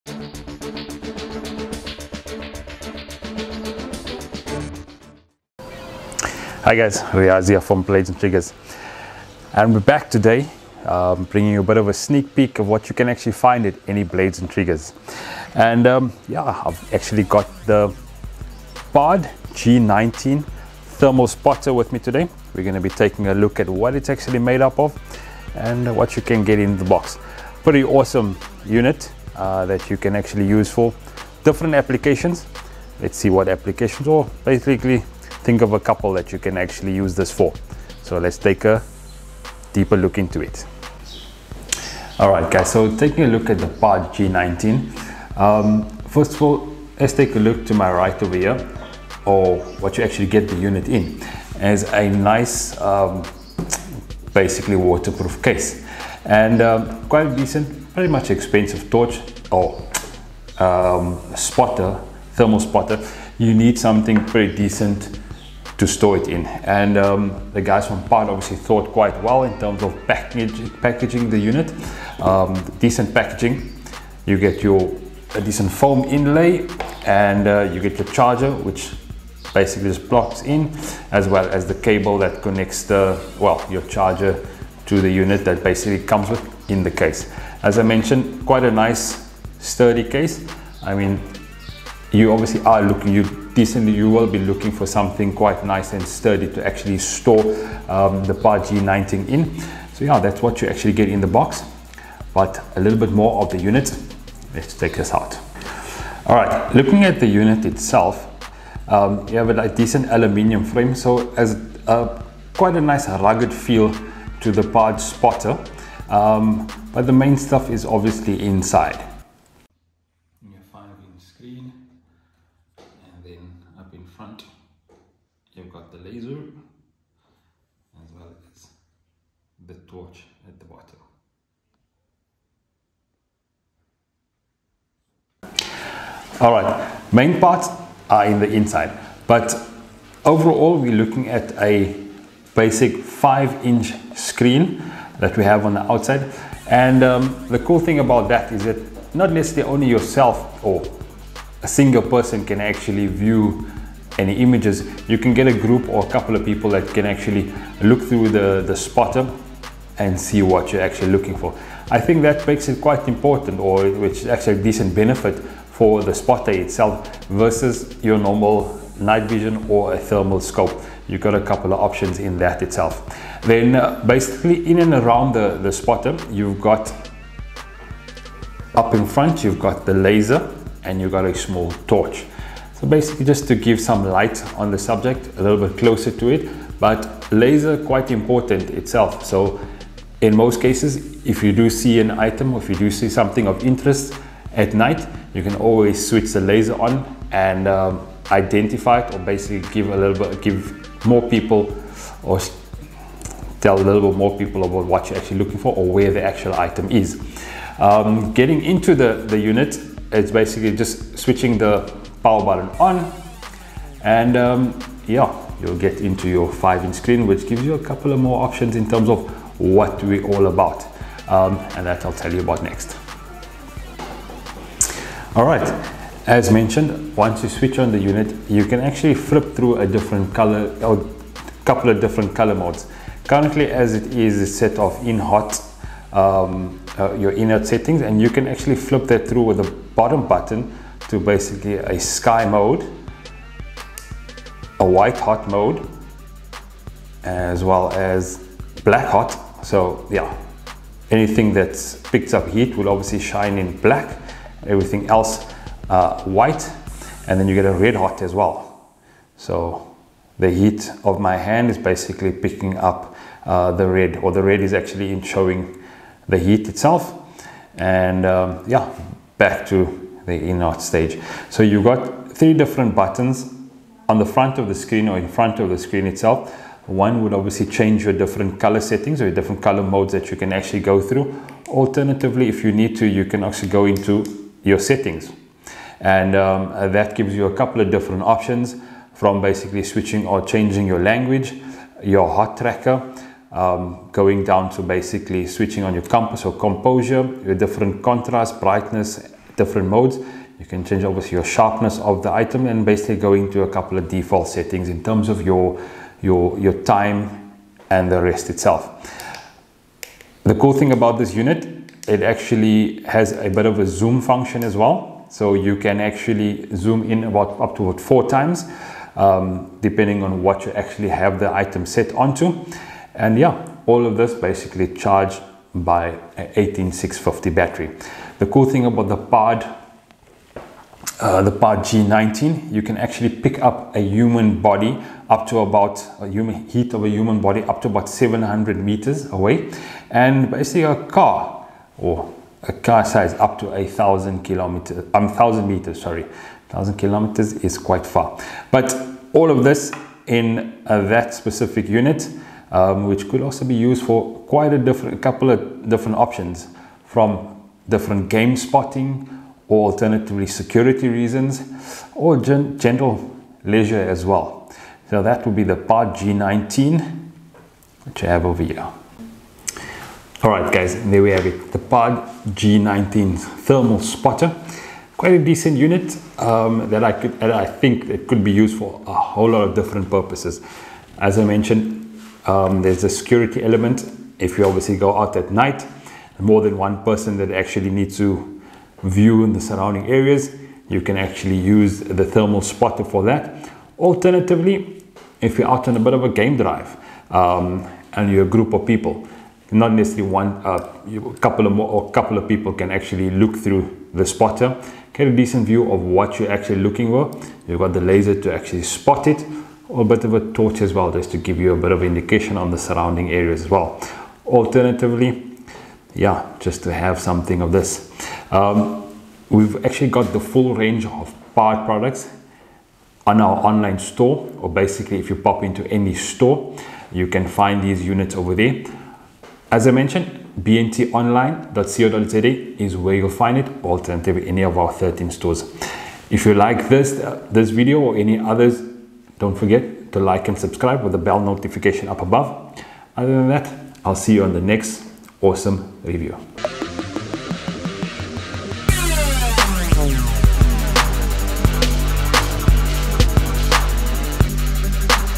Pard G19 Thermal Handheld Spotter Unboxing & Review
PARD G19 is an innovative handheld thermal detector, designed to locate and identify targets quickly and accurately. It has a large 5-inch LCD screen, which is ideal for indoor and outdoor use. The PARD has a built-in laser positioning system that can be used for precise measurement. The PARD is lightweight and compact, making it easy to carry and operate. It has a micro SD card that can store up to 300 images. This cool spotter is very reliable, as it is weather resistant and can be used in any weather conditions.

Timestamps

00:00 Introduction
00:41 What's in the Box
01:46 Unit features
02:18 Specifications
03:28 How to Operate
06:10 Uses
07:00 Where to Buy